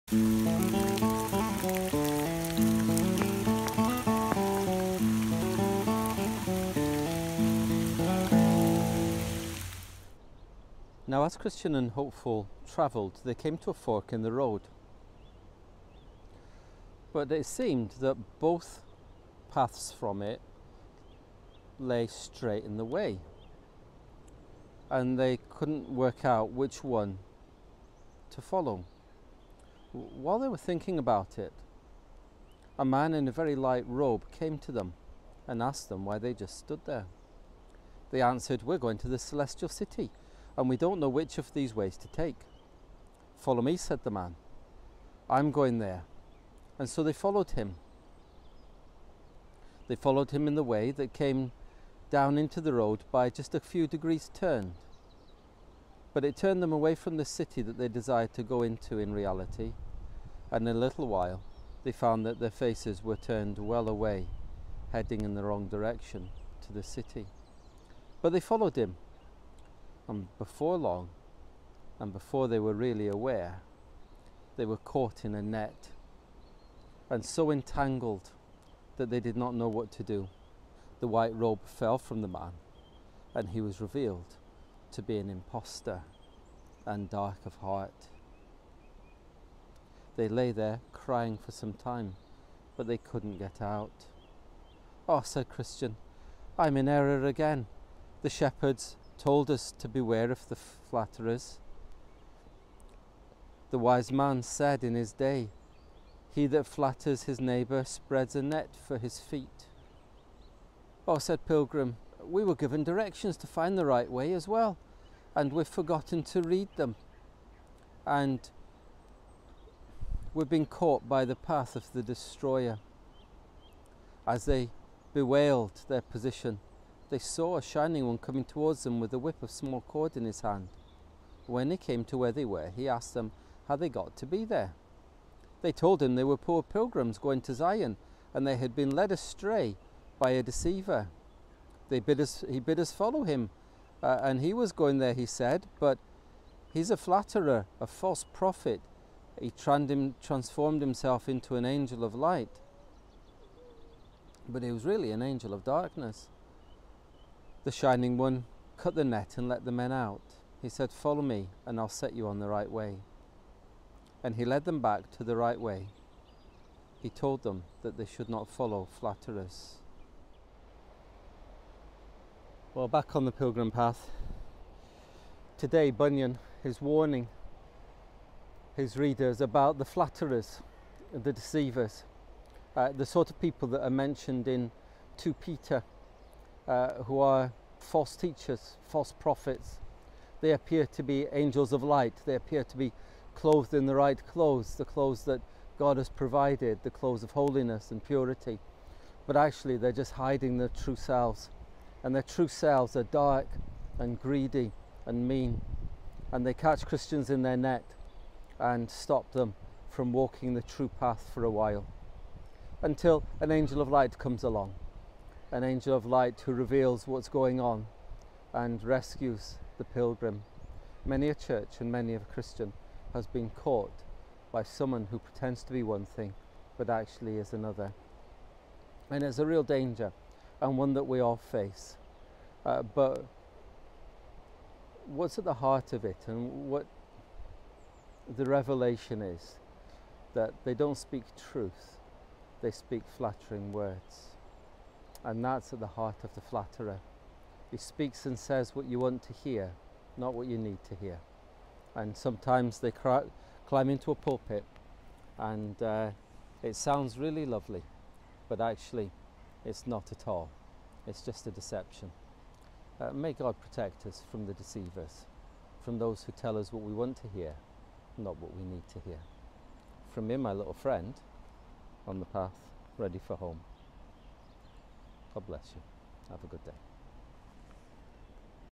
Pilgrim [44] Trapped by a flatterer
Jude 1:16
These people are grumblers and faultfinders; they follow their own evil desires; they boast about themselves and flatter others for their own advantage.

Stuart Dainty